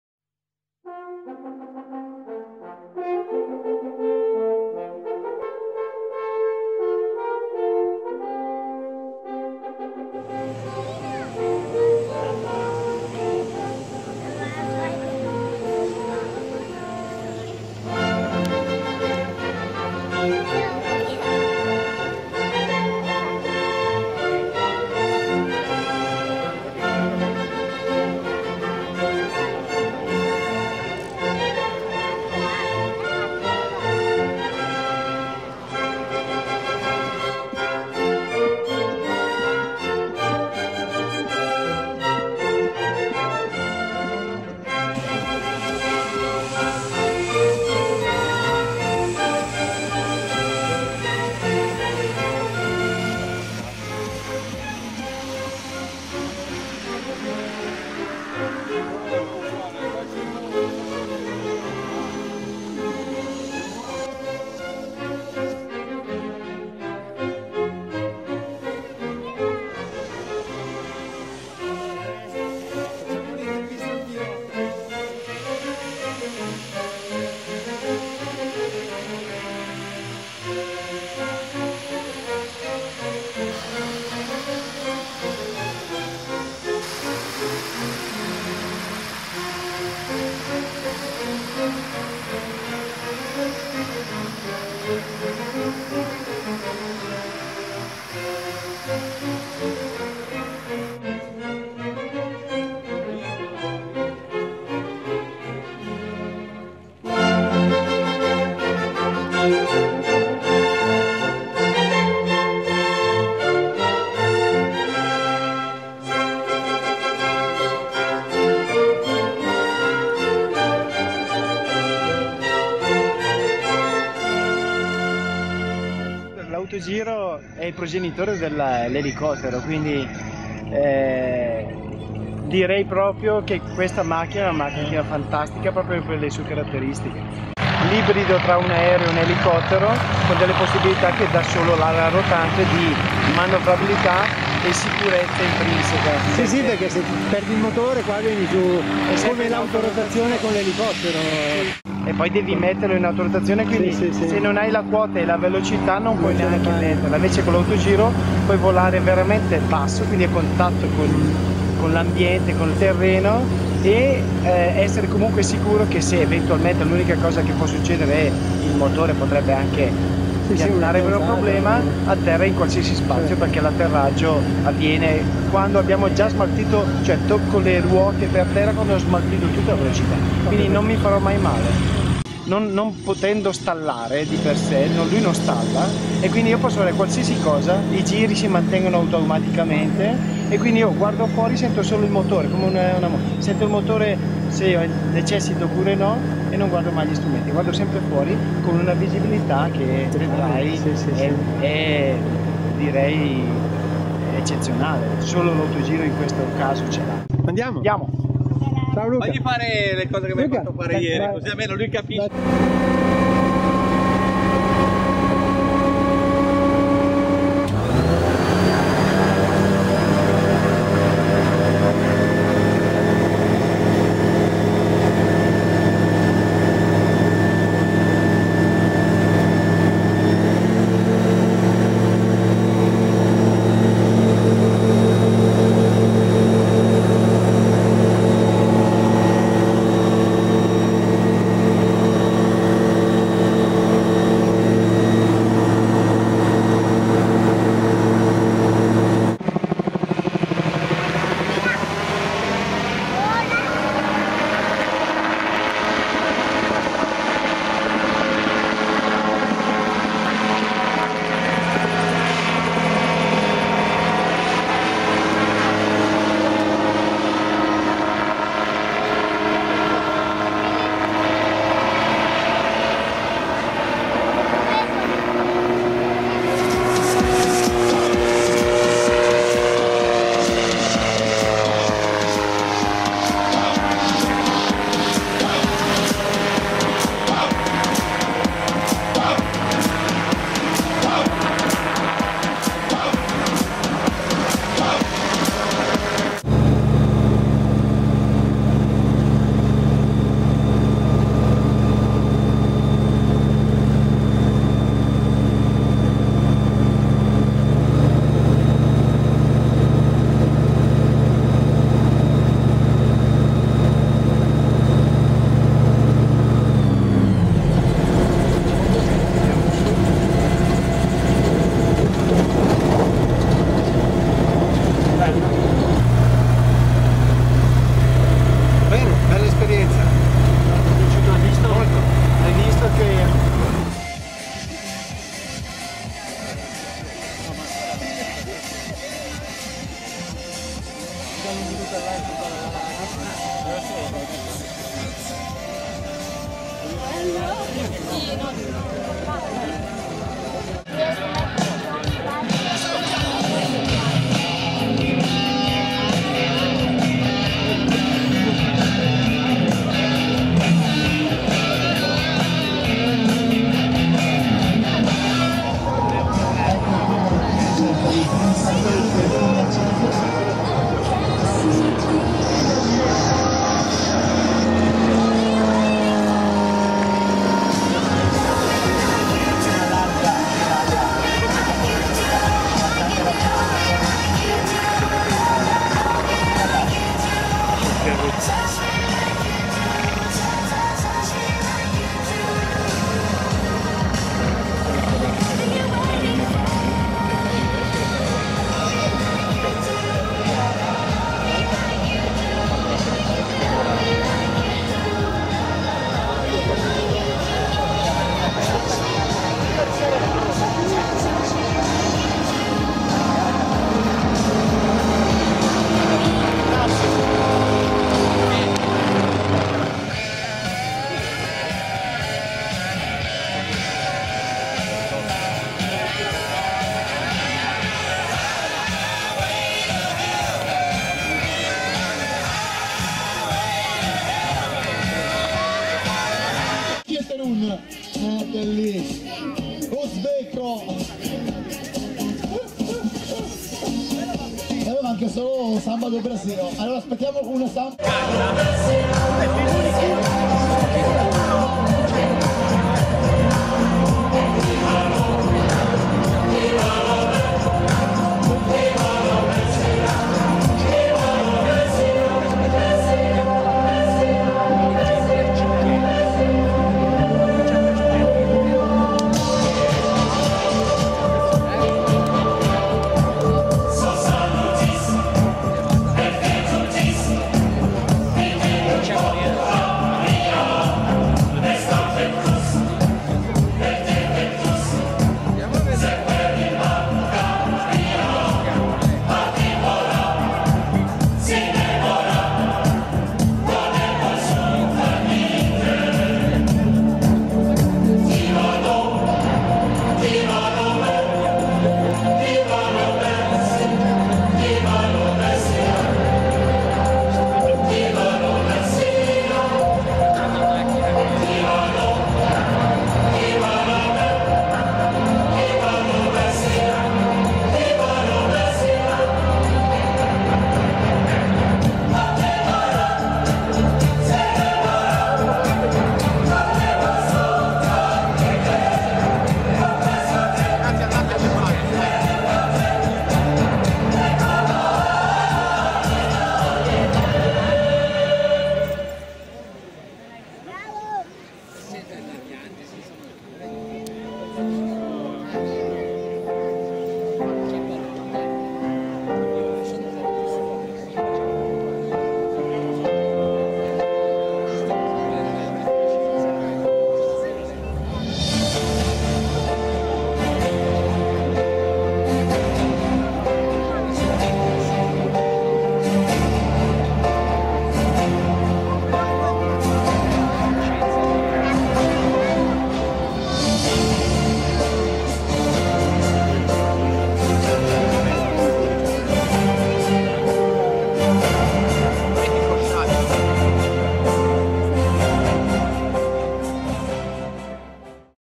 Festa dell'aria 2016 - Caiolo
Breve filmato a ricordo di una bellissima giornata trascorsa immerso nel mondo del volo.